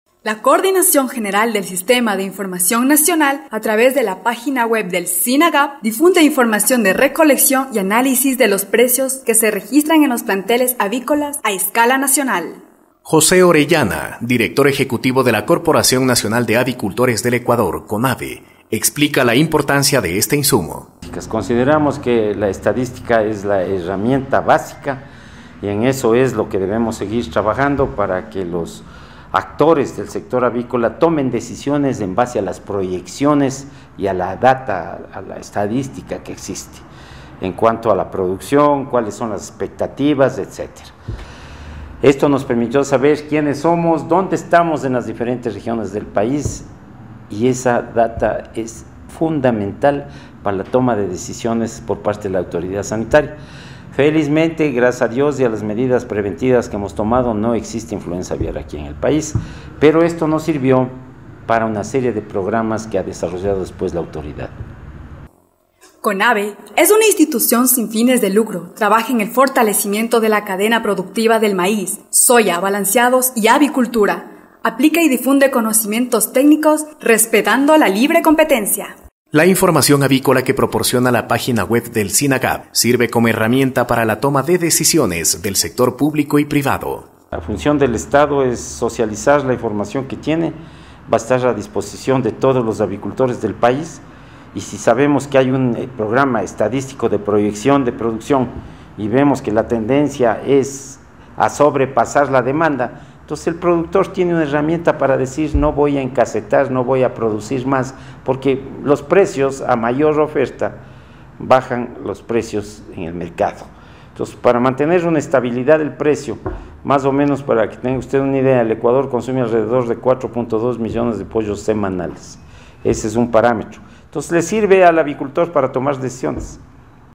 La información avícola que suministra el SINAGAP al sector privado.
La Coordinación General del Sistema de Información Nacional (CGSIN) a través de la página web del SINAGAP difunde información de recolección y análisis de los precios que se registran en los planteles avícolas a escala nacional.

CONAVE es una Institución sin fines de lucro que procura el fortalecimiento de la cadena productiva del maíz, soya, balanceados, y avicultura; aplicando y difundiendo conocimientos técnicos y respetando la libre competencia.

La información avícola que proporciona la página web del SINAGAP sirve como herramienta para la toma de decisiones del sector público y privado.